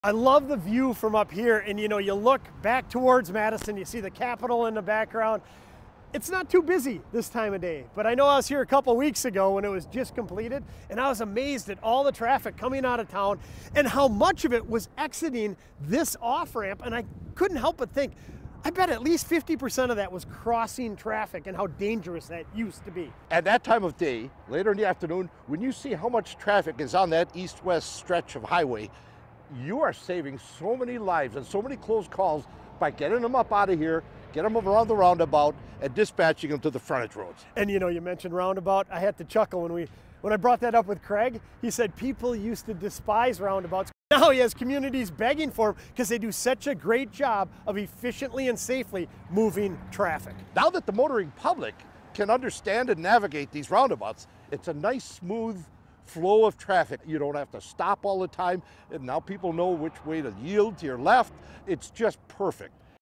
Perfect Roundabouts: Smooth Traffic Flow & Safety Explained
We explore how efficient and safe traffic management improves flow. Motoring public understanding leads to smoother navigation, eliminating constant stops. Our focus is on the perfect roundabout design, offering efficient traffic flow and understanding of yielding.